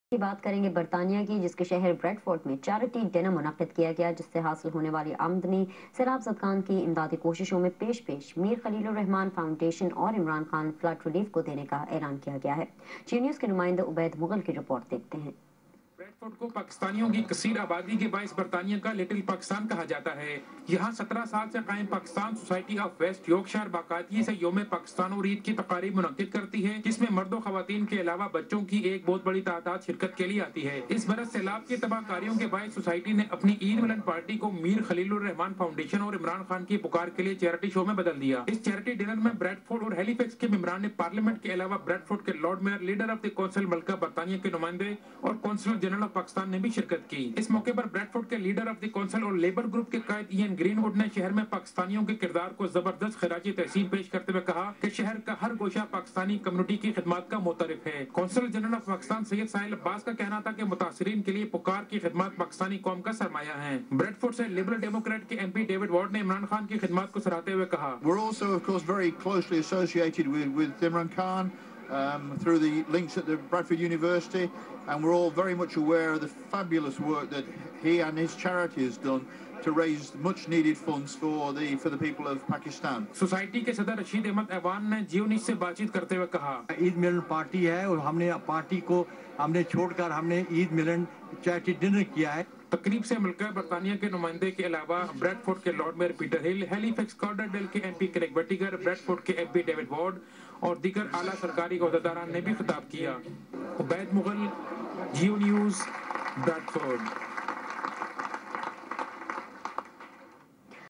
Geo Repoert Bradford by Ubaid Mughal
Charity Dinner and Eid party organised by Pakistan society of west yorkshire. money donated to Imrank Khan ad Mir Khalil ur Rehman Foundation